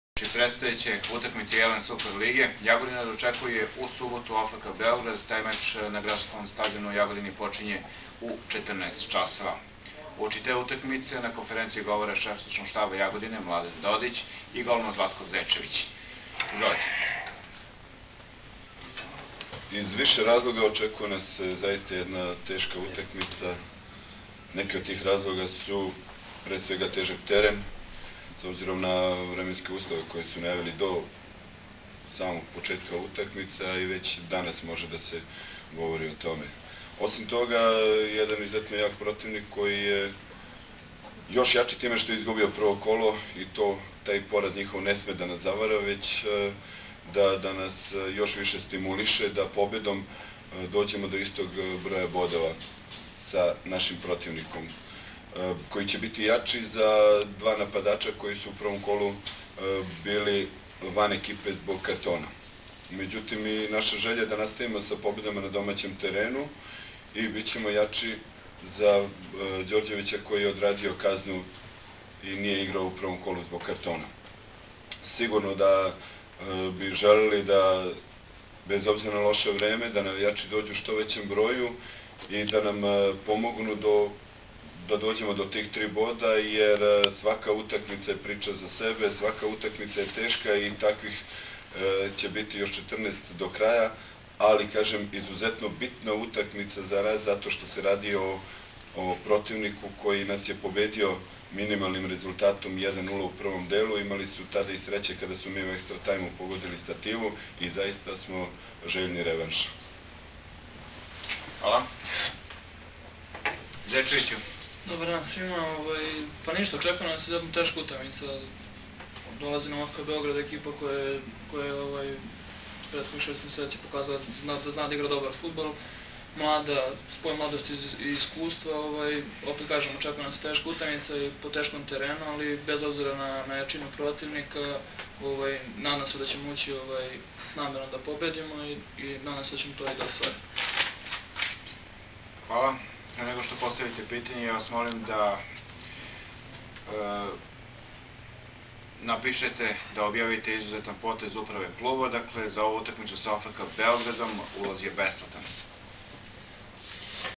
Konferencija za stampu 04.03.2010.
Konferencija za štampu povodom otvaranja prolećne polusezone na Gradskom stadionu. Govore šef stručnog štaba Mladen Dodić I golman Zlatko Zečević.